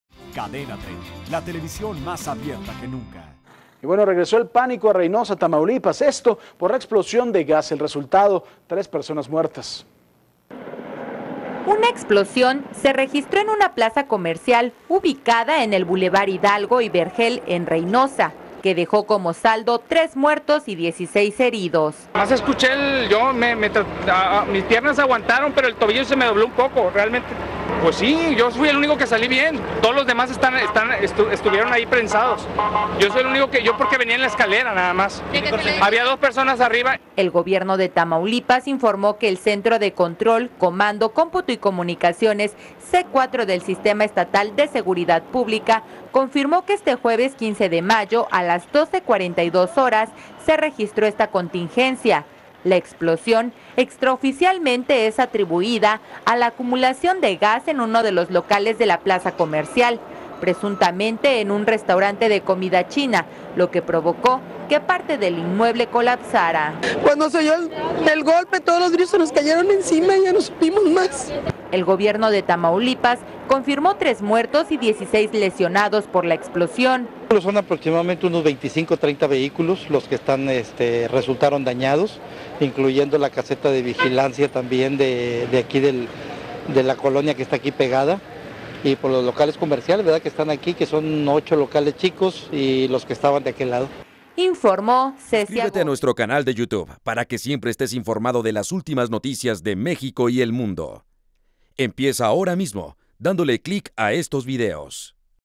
Explosión en una plaza comercial de Reynosa / Explosion in a commercial center of Reynosa
Explosión en una plaza comercial de Reynosa, Tamaulipas / Explosion in a commercial center of Reynosa, Tamaulipas

16 de mayo 2014.